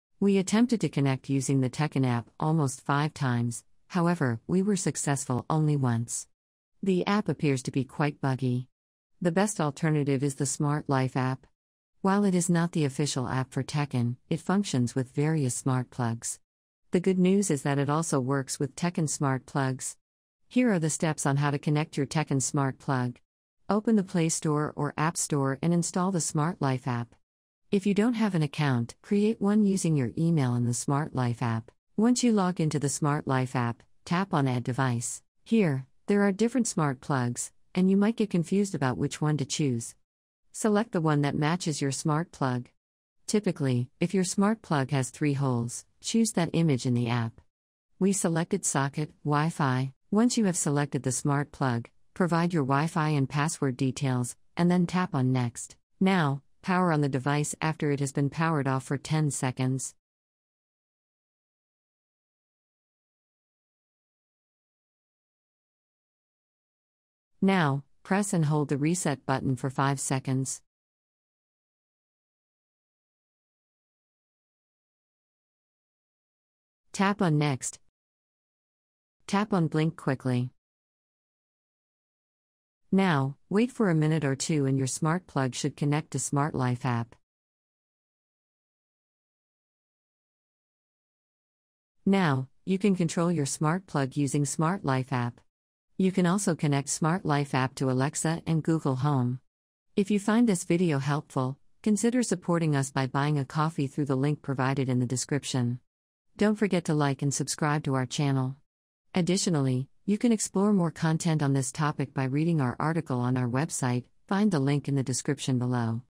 How to Fix Teckin Smart Plug Not Connecting
Welcome to the step-by-step guide on how to fix connectivity issues with your Teckin Smart Plug using the Smart Life app. If you've struggled with the Teckin mobile app and faced difficulties connecting, worry not – we'll walk you through a seamless process using Smart Life.